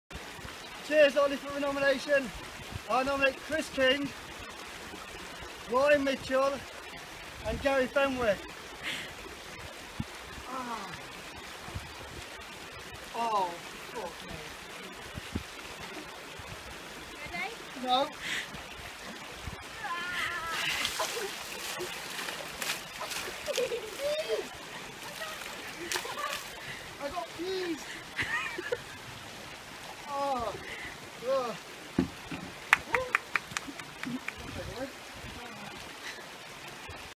Pea Ice Bucket Challenge
Decided to go into the peaks for a more rural experience